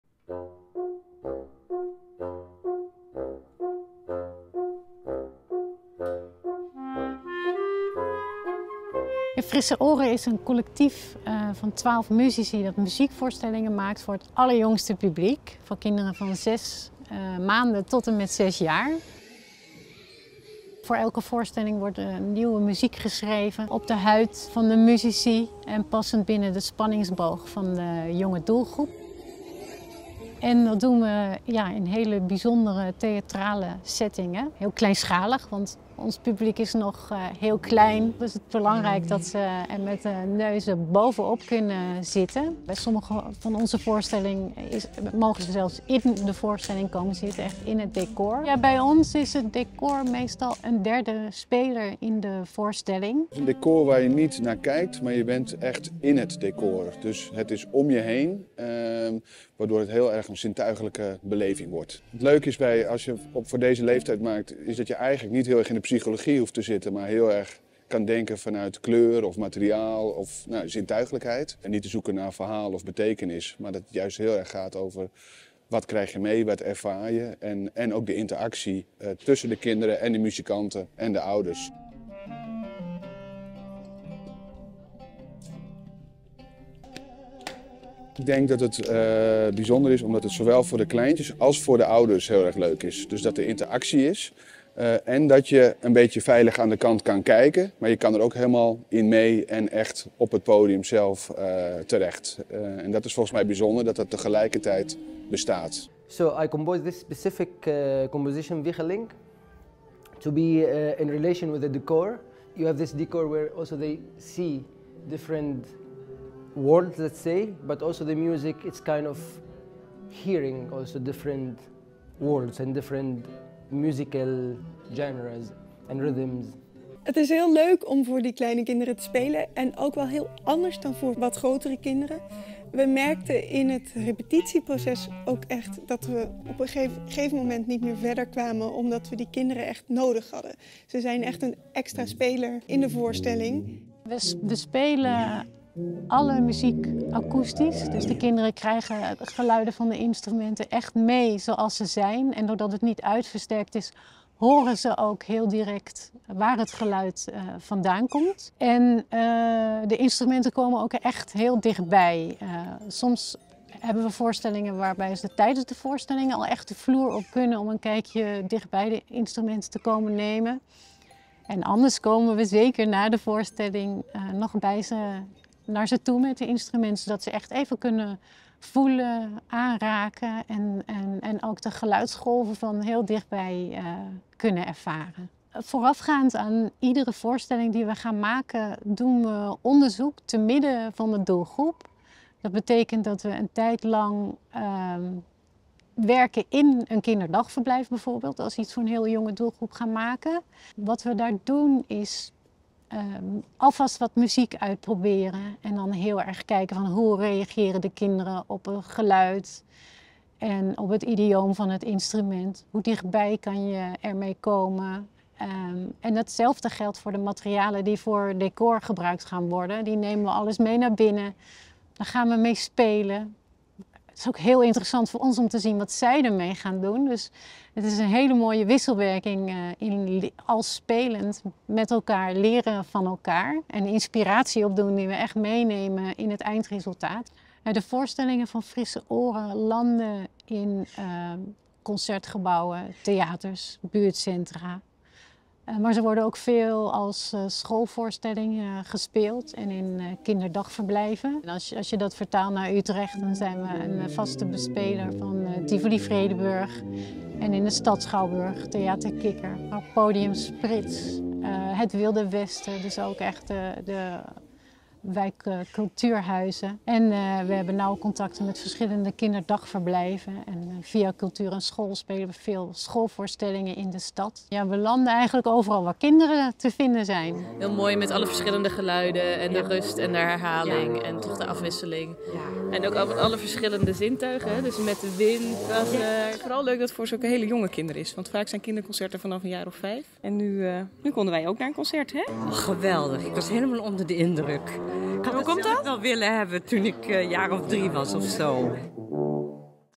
Over Frisse Oren
Frisse Oren wil zo veel mogelijk kinderen in de leeftijd van 6 maanden tot 6 jaar, live gespeelde en nieuw geschreven muziek laten beleven in theater, concert- zaal, buurtcentrum of school. Met poëtische en tegendraadse muziekvoorstellingen die zowel kinderen als hun (groot)ouders raken. De voorstellingen van Frisse Oren zijn altijd tekstloos. Dat maakt ze toegankelijk voor iedereen!

Met o.a. beelden van Dirk Kruyder/Camont media i.o.v. Muziekgebouw aan 't IJ